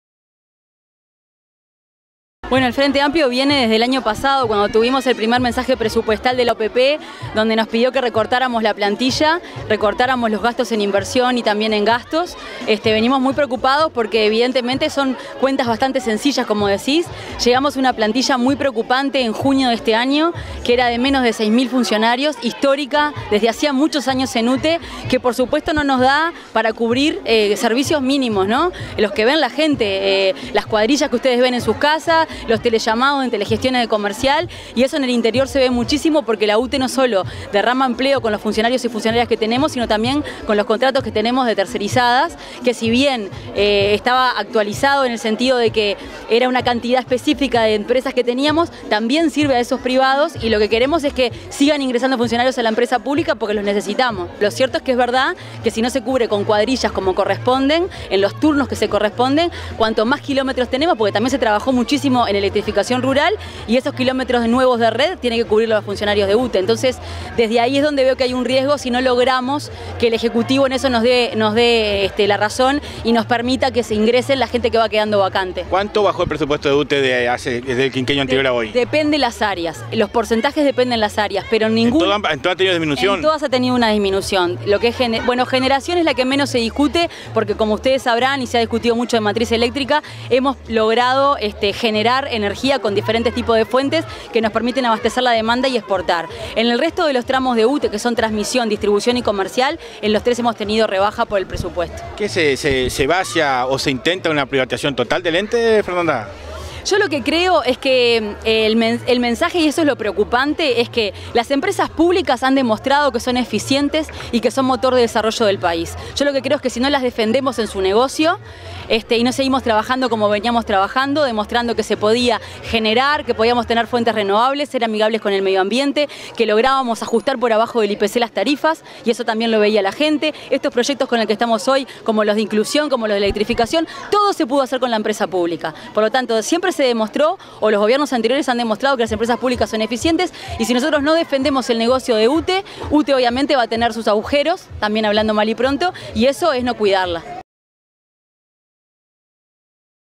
Fernanda Cardona- Preocupan recortes presupuestales y de personal en UTE
Y sobre la gestión de UTE, Fernanda Cardona, integrante del directorio del ente, planteo la preocupación de su sector político por los recortes que se han dado en la empresa pública y la falta de personal, generando cuadrillas que no tienen la capacidad de respuestas ante tantos reclamos.